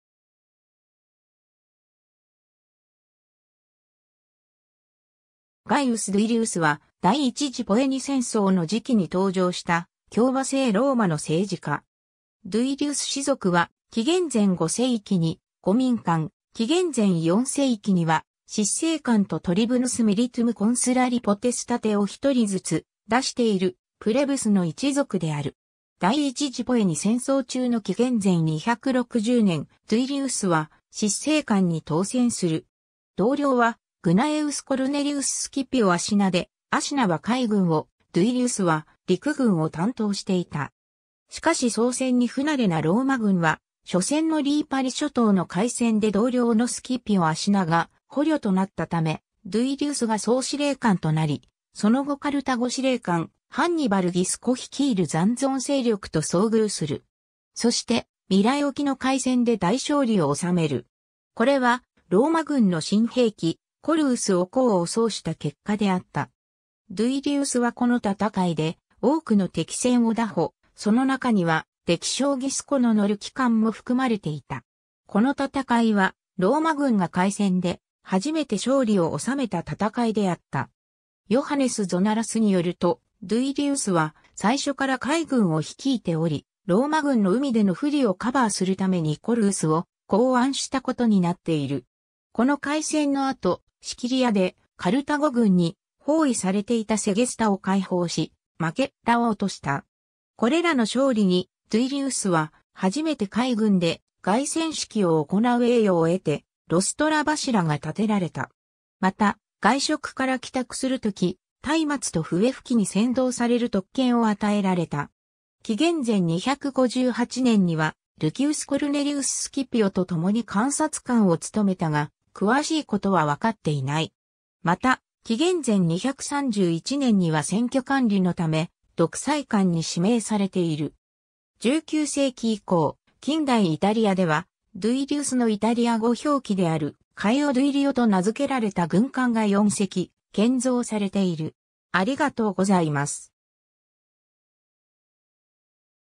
ガイウス・ドゥイリウス
ガイウス・ドゥイリウス（ラテン語: Gaius Duilius, 紀元前3世紀、生没年不詳）は、第一次ポエニ戦争の時期に登場した共和政ローマの政治家。
 ドゥイリウス氏族は紀元前5世紀に護民官、紀元前4世紀には執政官とトリブヌス・ミリトゥム・コンスラリ・ポテスタテを一人ずつ出している、プレブスの一族である。
 第一次ポエニ戦争中の紀元前260年、ドゥイリウスは執政官に当選する。
同僚はグナエウス・コルネリウス・スキピオ・アシナで、アシナは海軍を、ドゥイリウスは陸軍を担当していた。
しかし操船に不馴れなローマ軍は、緒戦のリーパリ諸島の海戦で同僚のスキピオ・アシナが捕虜となったため、ドゥイリウスが総司令官となり、その後カルタゴ司令官ハンニバル・ギスコ率いる残存勢力と遭遇する。
そしてミラエ沖の海戦で大勝利を収める。
これはローマ軍の新兵器「コルウス」（カラス）を功を奏した結果であった。
ドゥイリウスはこの戦いで多くの敵船を拿捕、その中には敵将ギスコの乗る旗艦も含まれていた。
この戦いはローマ軍が海戦で初めて勝利を収めた戦いであった。
ヨハネス・ゾナラスによると、ドゥイリウスは最初から海軍を率いており、ローマ軍の海での不利をカバーするためにコルウスを考案したことになっている。
 この海戦の後、シキリアでカルタゴ軍に包囲されていたセゲスタを解放し、マケッラを落とした。
これらの勝利にドゥイリウスは初めて海軍で凱旋式を行う栄誉を得て、ロストラ柱が建てられた。
また、外食から帰宅する時、松明と笛吹きに先導される特権を与えられた。
 紀元前258年にはルキウス・コルネリウス・スキピオと共に監察官（ケンソル）を務めたが、詳しい事は分かっていない。
また、紀元前231年には選挙管理の為独裁官に指名されている。
 19世紀以降、近代イタリアではドゥイリウスのイタリア語表記である「カイオ・ドゥイリオ（イタリア語: Caio Duilio）」と名付けられた軍艦が4隻建造されている。